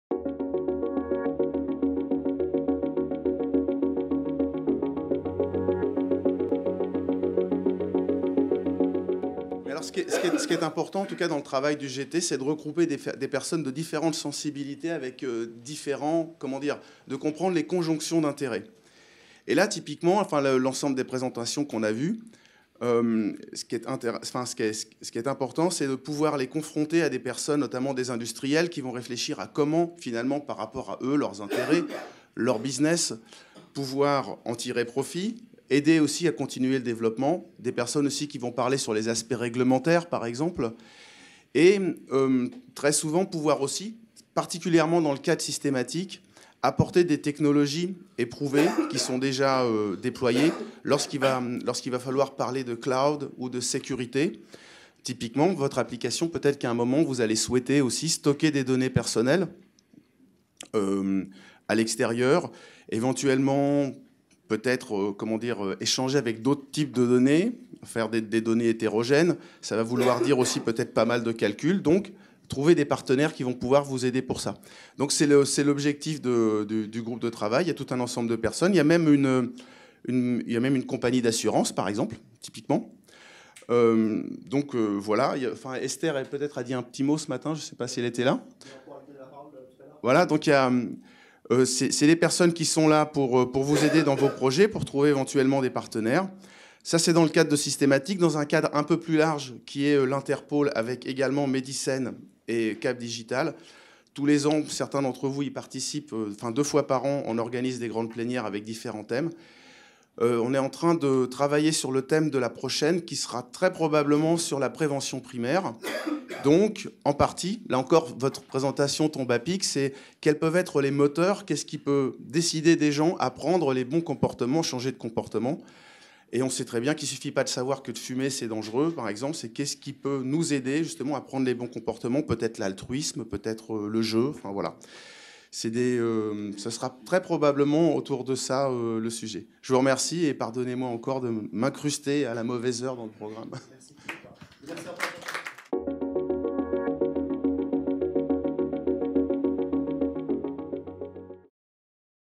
Journée E-Santé Paris-Saclay - Philippe Gesnouin
Présenté par Philippe Gesnouin lors de la Journée E-Santé Paris-Saclay, le vendredi 8 décembre 2017